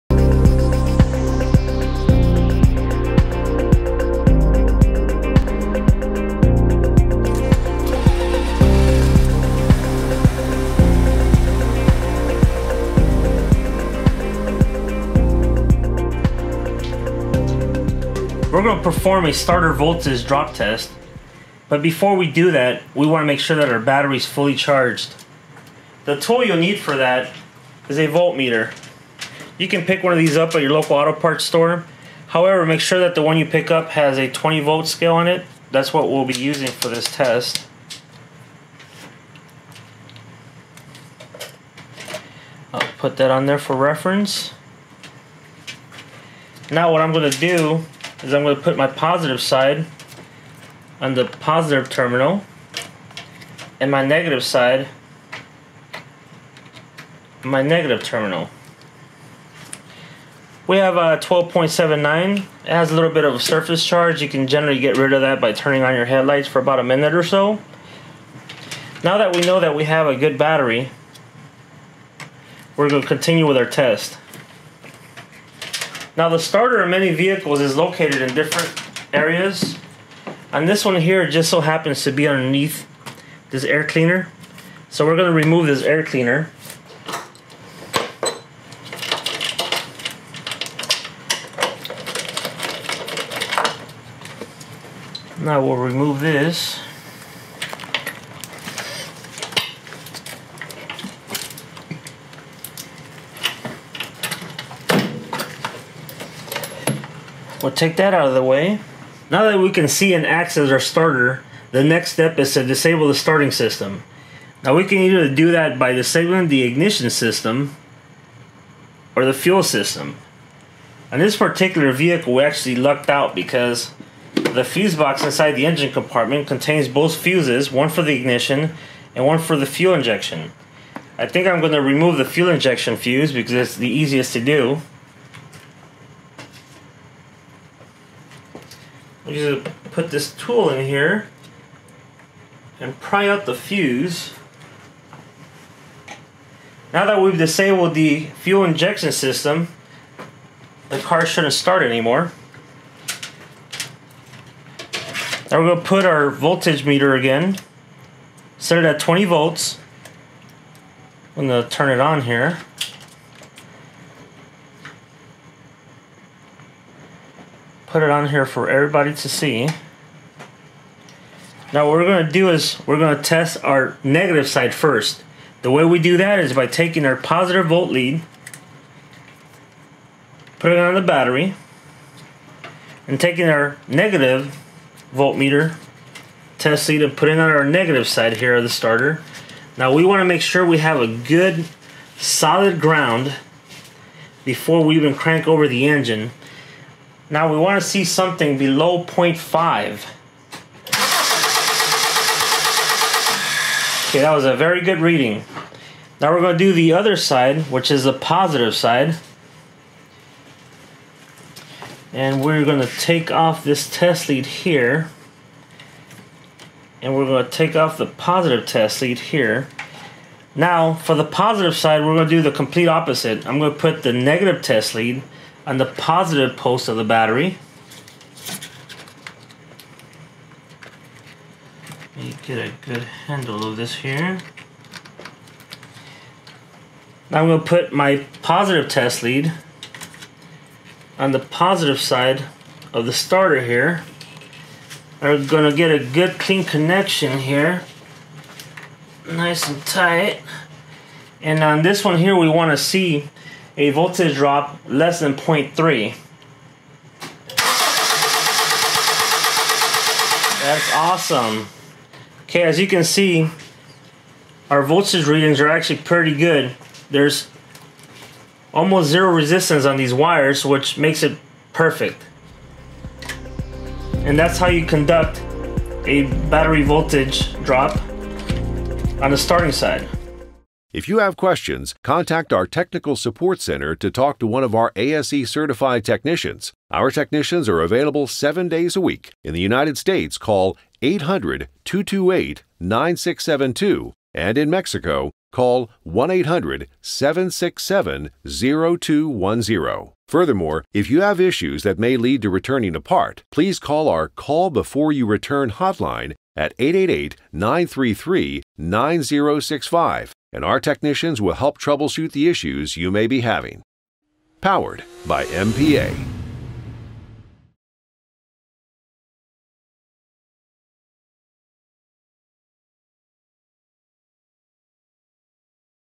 Voltage Drop Testing the Starter Circuit
This new video from MPA will show you the process for properly testing the starter circuit. The test in this video is being performed on a Honda CR-V.